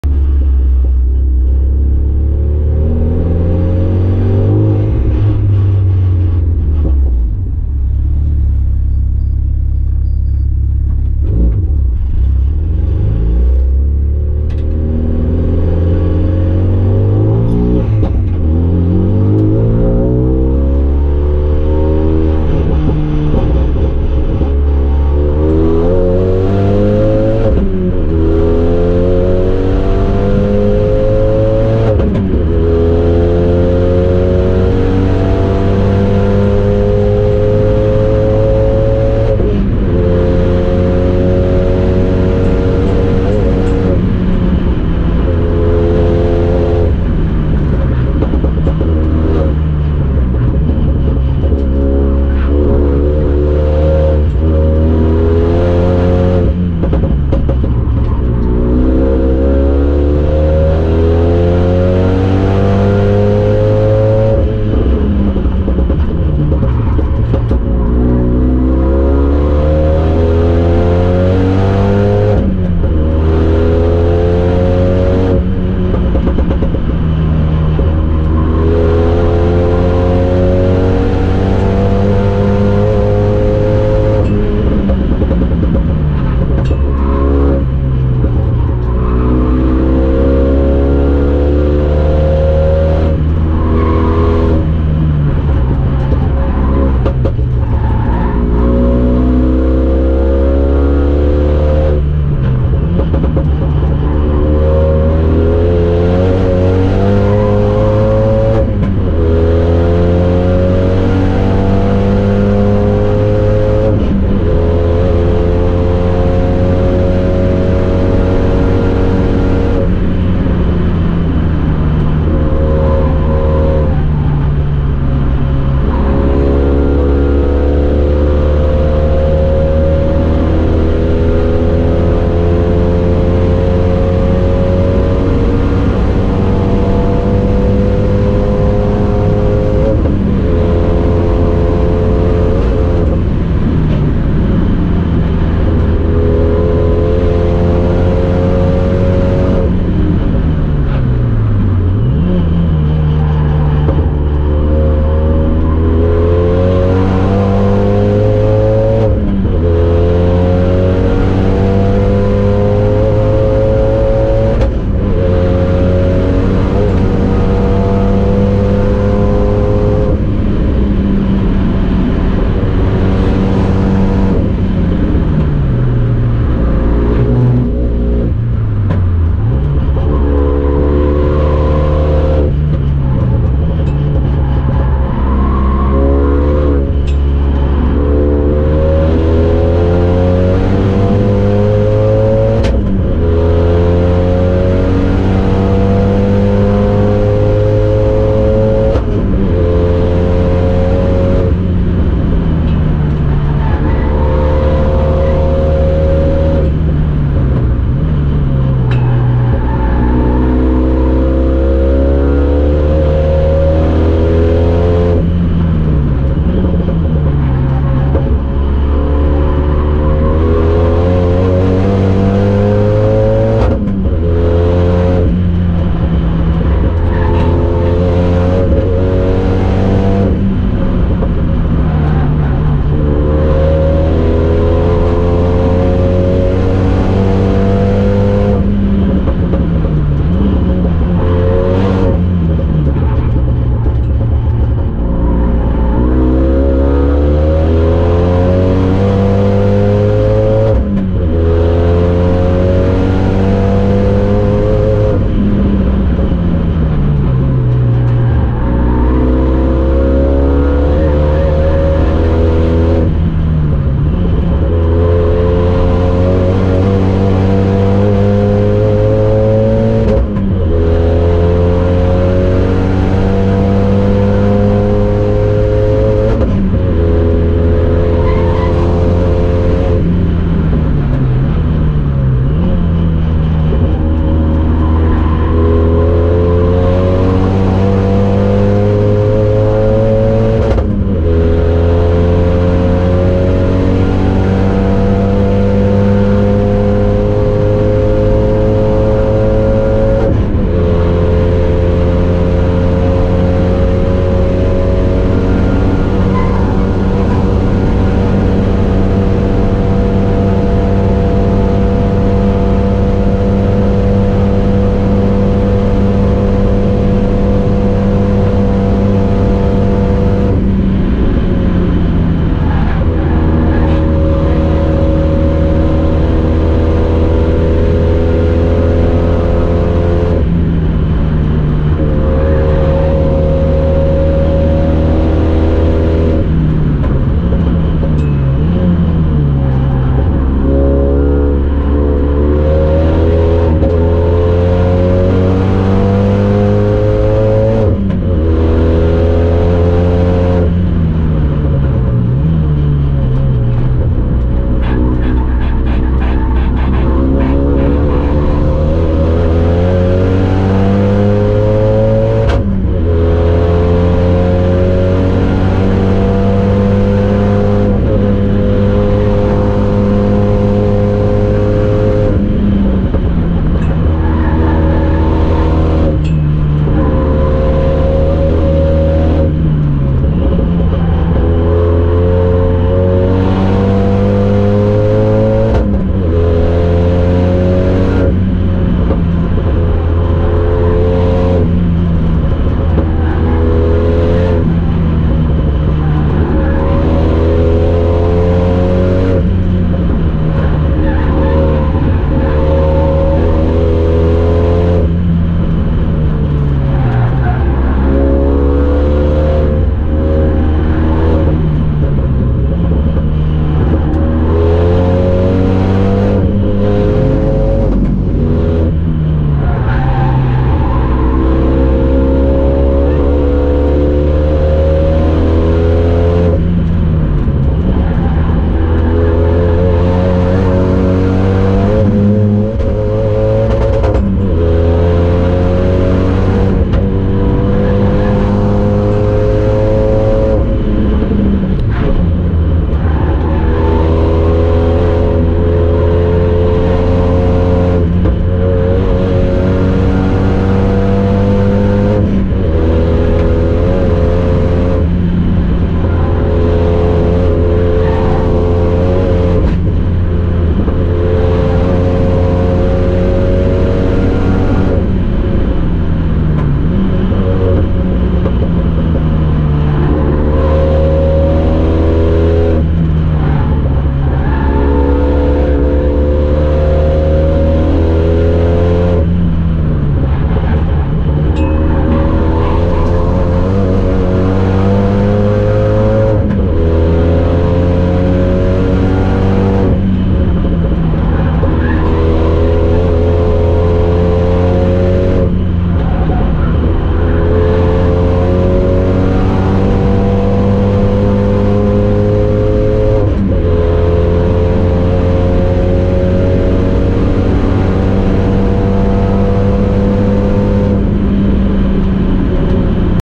BTG 7.52 - Fahrbach Motorsport Tracktool - 30.09.2018 Nordschleife Touristenfahrten
Renault Megane 3 RS Trophy 265.

1217kg
Software STOCK!
Wiechers Roll Cage
Japan Racing Wheels with 235/40 18 Pirelli Trofeo R
Drexler Limited Slip Diff
CAE Racing Short-Shifter
Sabelt R26.R Seats + Safety Belts
RSC Suspension